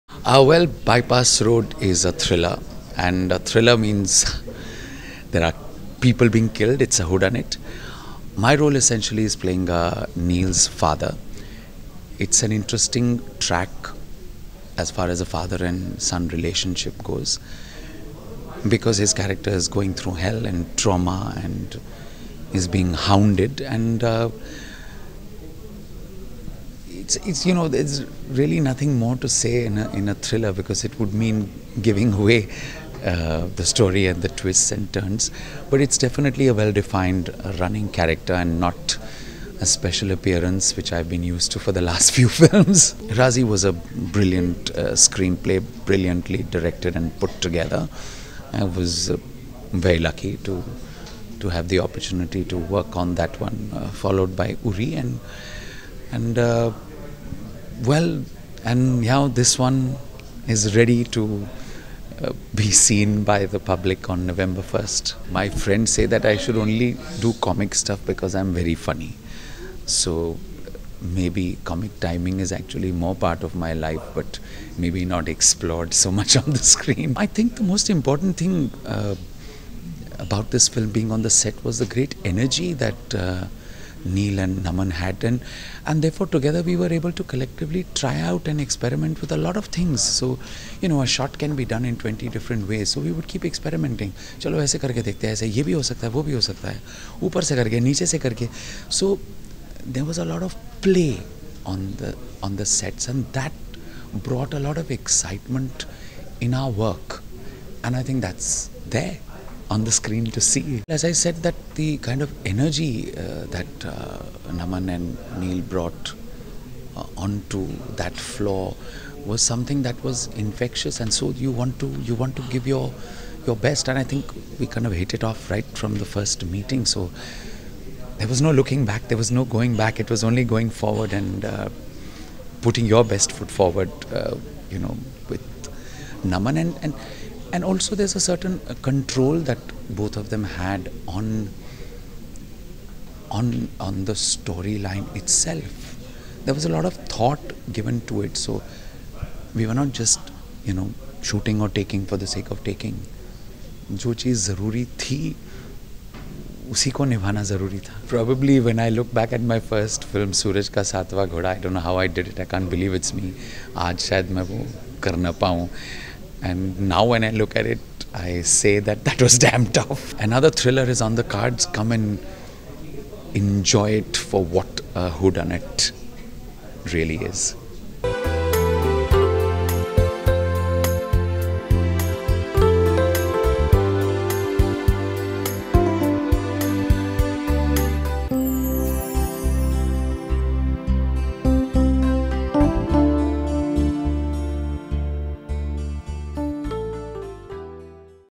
Veteran Actor Rajit Kapur Reveals The Reason Behind Working In Thriller Movies | LehrenTV
In an interview with Rajit Kapur, the actor talks about his upcoming movie Bypaas Road and he also reveals the reason behind choosing thriller movies. Check out this candid  interview to know more.